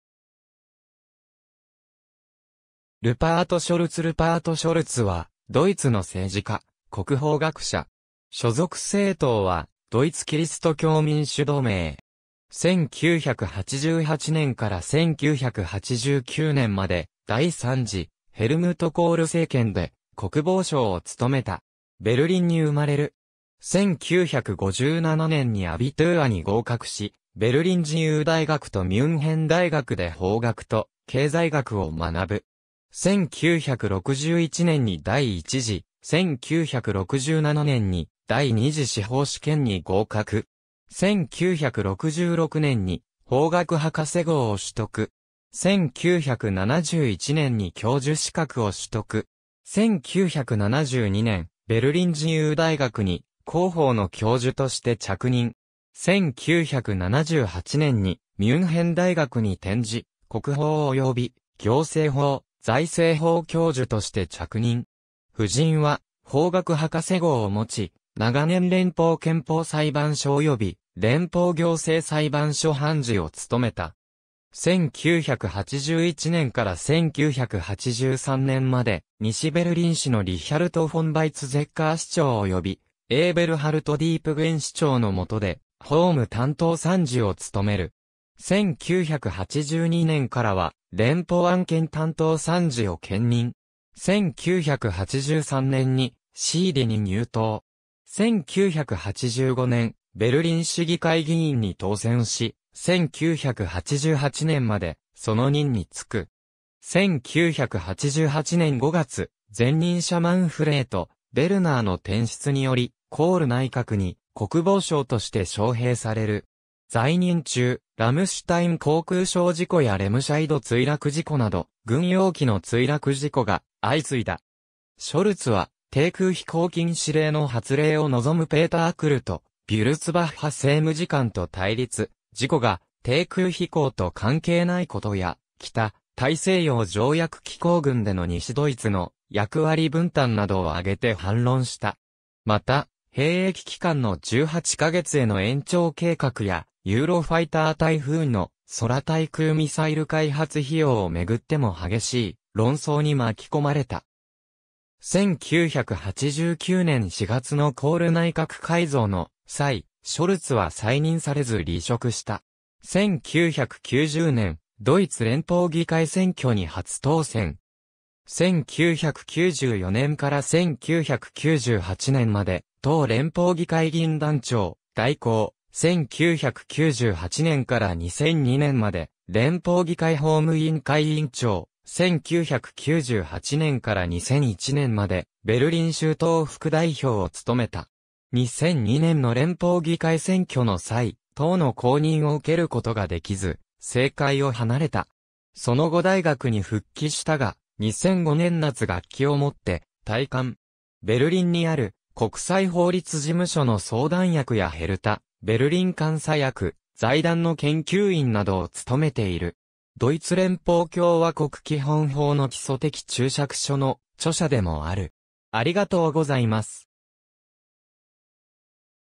ルパート・ショルツ
ルートヴィヒ・マクシミリアン大学ミュンヘンの教員
ルートヴィヒ・マクシミリアン大学ミュンヘン出身の人物
ルパート・ショルツ（1988年） ルパート・ショルツ（Rupert Scholz, 1937年5月23日 - ）は、ドイツ（西ドイツ）の政治家、国法学者。
所属政党はドイツキリスト教民主同盟 (CDU)。
1988年から1989年まで、第3次ヘルムート・コール政権で国防相を務めた。
ベルリンに生まれる。
1957年にアビトゥーアに合格し、ベルリン自由大学とミュンヘン大学で法学と経済学を学ぶ。
1961年に第一次、1967年に第二次司法試験に合格。
1966年に法学博士号を取得。
1971年に教授資格を取得。
1972年、ベルリン自由大学に公法の教授として着任。
1978年にミュンヘン大学に転じ、国法および行政法、財政法教授として着任。
夫人は法学博士号をもち、長年連邦憲法裁判所および連邦行政裁判所判事を務めた。
1981年から1983年まで、西ベルリン市のリヒャルト・フォン・ヴァイツゼッカー市長およびエーベルハルト・ディープゲン市長の下で法務担当参事を務める。
1982年からは連邦案件担当参事を兼任。
1983年にCDUに入党。
1985年、ベルリン市議会議員に当選し、1988年までその任に就く。
1988年5月、前任者マンフレート・ヴェルナーの転出により、コール内閣に国防相として招聘される。
在任中ラムシュタイン航空ショー事故やレムシャイド墜落事故など、軍用機の墜落事故が相次いだ。
ショルツは低空飛行禁止令の発令を望むペーター・クルト・ヴュルツバッハ政務次官と対立、事故が低空飛行と関係ないことや、北大西洋条約機構軍での西ドイツの役割分担などを挙げて反論した。
また兵役期間の18か月への延長計画やユーロファイター タイフーンの空対空ミサイル開発費用を巡っても激しい論争に巻き込まれた。
1989年4月のコール内閣改造の際、ショルツは再任されず離職した。
1990年、ドイツ連邦議会選挙に初当選。
1994年から1998年まで党連邦議会議員団長代行、1998年から2002年まで連邦議会法務委員会委員長、1998年から2001年までベルリン州党副代表を務めた。
2002年の連邦議会選挙の際、党の公認を受けることができず、政界を離れた。
その後大学に復帰したが、2005年夏学期を以て退官。
ベルリンにある国際法律事務所の相談役やヘルタ・ベルリン監査役、財団の研究員などを務めている。
ドイツ連邦共和国基本法の基礎的注釈書の著者でもある。